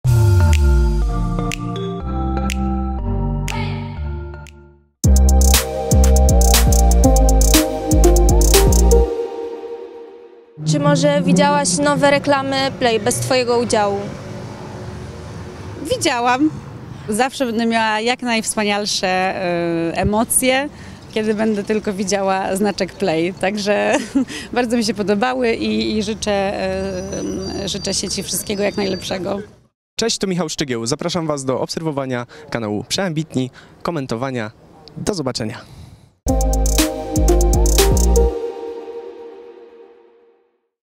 BASIA Kurdej-Szatan o nowych REKLAMACH PLAY bez jej udziału | przeAmbitni.pl
Wywiad z Basia Kurdej-Szatan, Philipp Plain, Warszawa -  02.06.22 r.